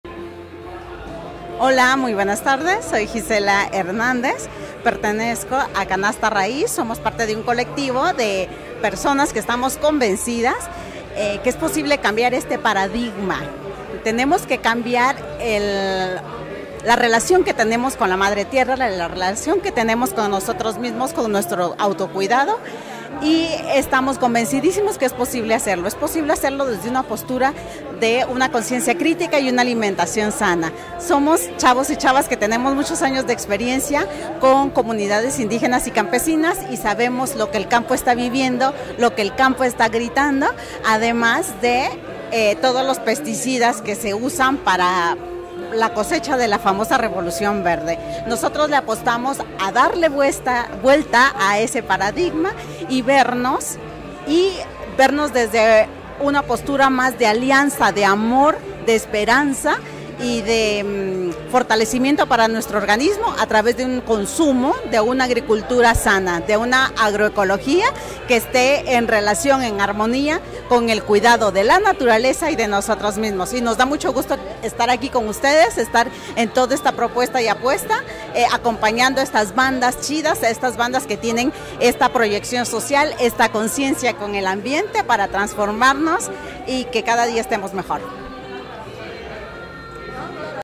PISTAV8ESYMAS, CANASTA RAIZ ¡QUÉ ALEGRÍA QUE SEAS PARTE DE NUESTRA COMUNIDAD!
Te queremos compartir nuestro SUEÑO y nuestra HISTORIA, para que puedas valorar lo que existe detrás de cada uno de los productos de Canasta Raíz. Nos encantaría que la leyeras toda. Es un poquito larga, así que bien valdría prepararte un café (de preferencia orgánico) y acomodarte en tu sillón favorito.

Facebook Instagram You tube